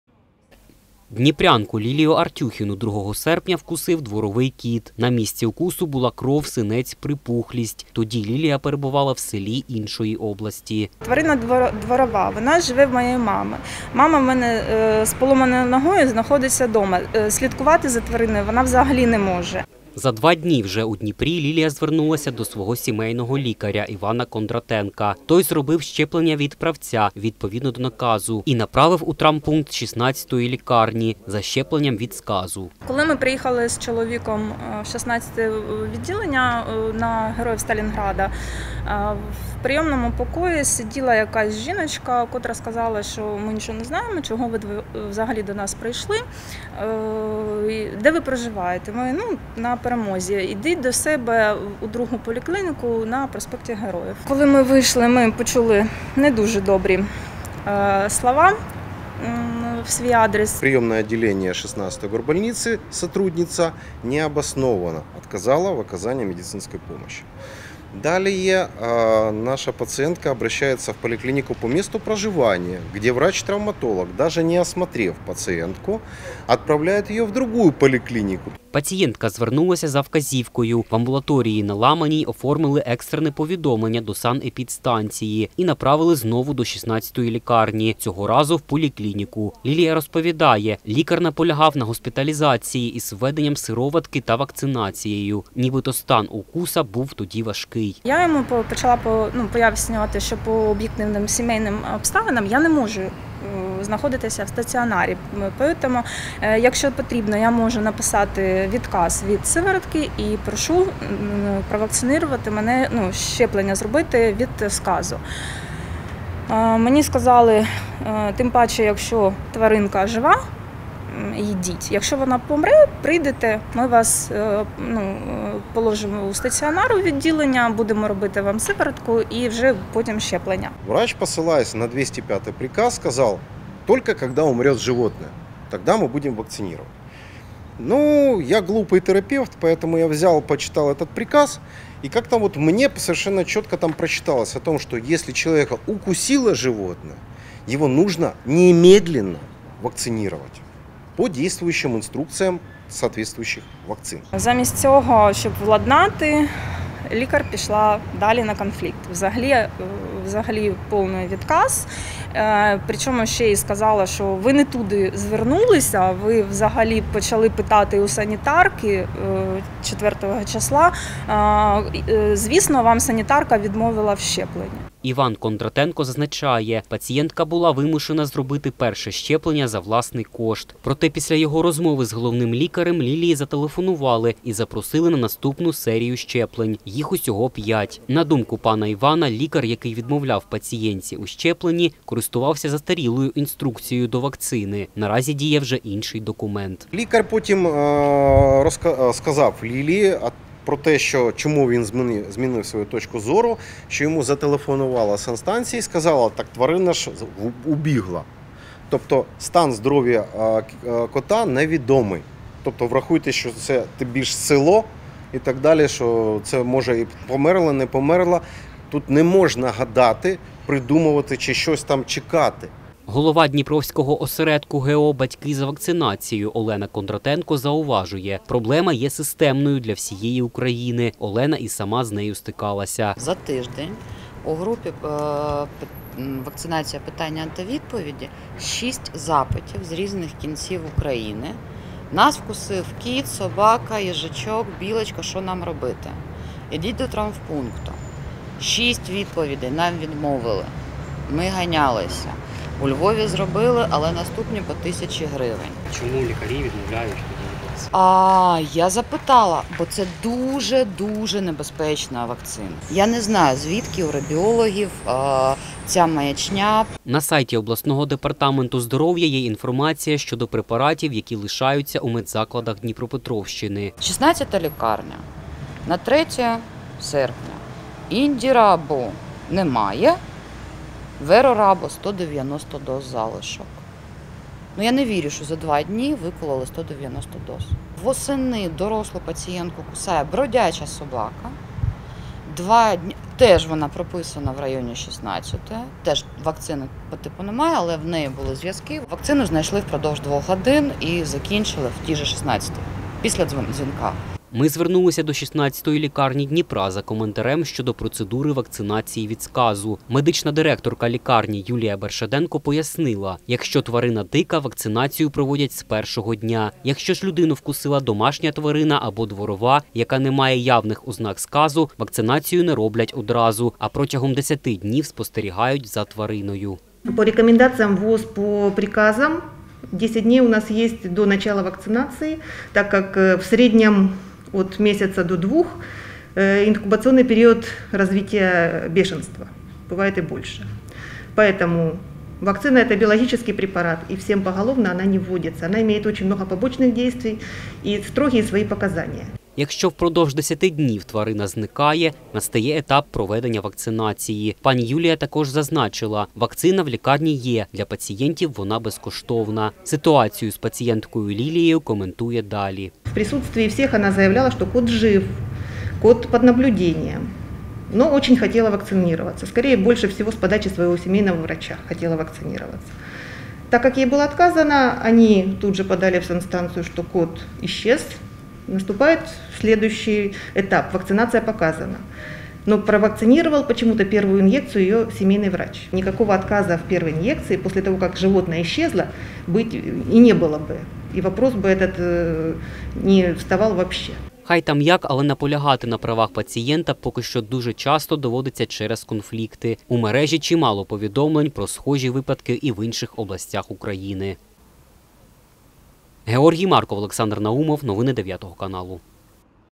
Медична система України: все складно. Як дніпрянка добивалася щеплення від сказу.
Що робити, коли людину вкусила тварина? Ясна річ -  звернутися до лікарів. Але не поспішайте,  не все так просто. Героїні наступного сюжету довелося обійти кілька медичних закладів, посваритися з лікарями, і лише після цього отримати щеплення від сказу. Якою сьогодні є процедура непланової вакцинації і на яких підставах медики можуть відмовити у щепленні — розбиралися наші журналісти.